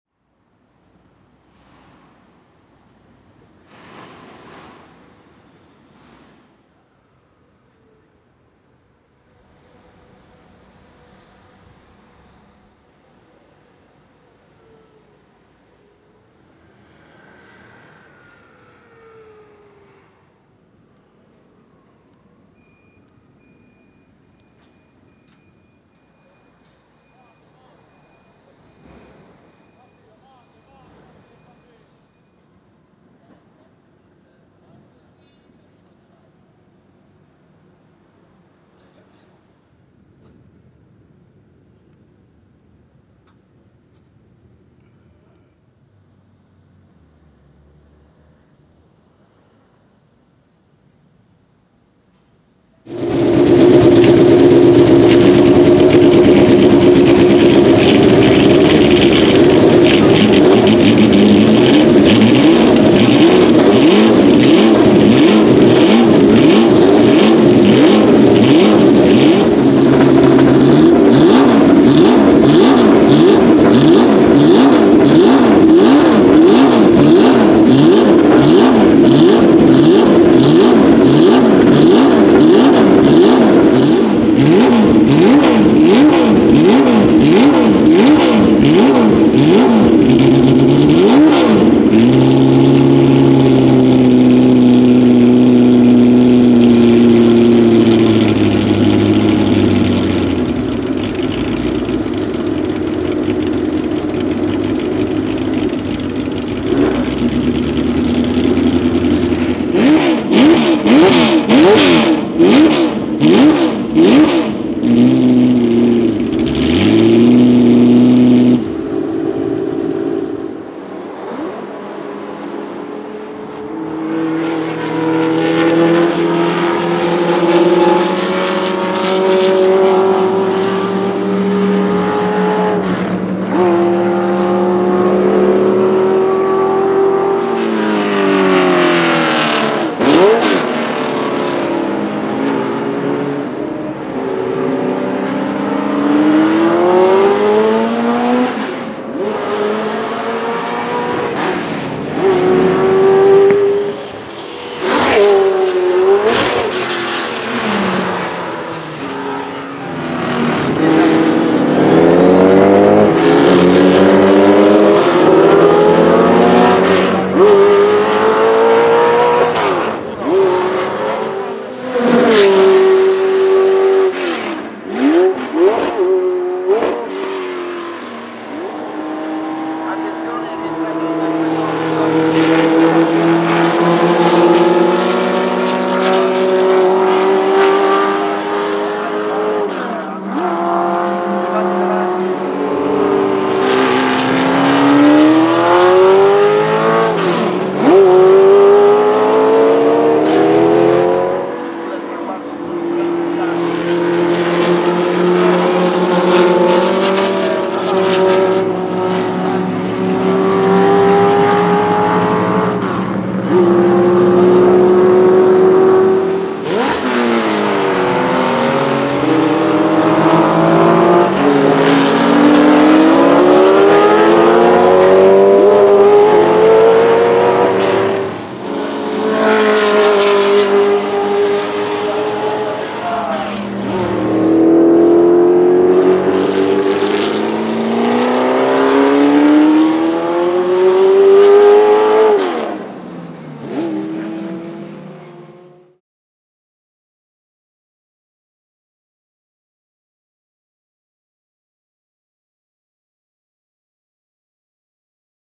Porsche 908/3 Flat-8 Engine Sound - Warm Up & In Action On Track
The Porsche 908/3 of this video is chassis 001 which was used by Porsche as test and development car. It's now owned and driven by Peter Vögele.
The car is moved by a 3.0 litre flat-8 engine which was able to produce more than 350 bhp at 8400 rpm and 365 Nm of torque at 6600 rpm with a weight of only 540 kg / 1190 lbs.

Camcorder: Canon Legria HF G30 + Canon DM-100 Microphone
Event: Monza Historic 2015
Where: Autodromo di Monza, Italy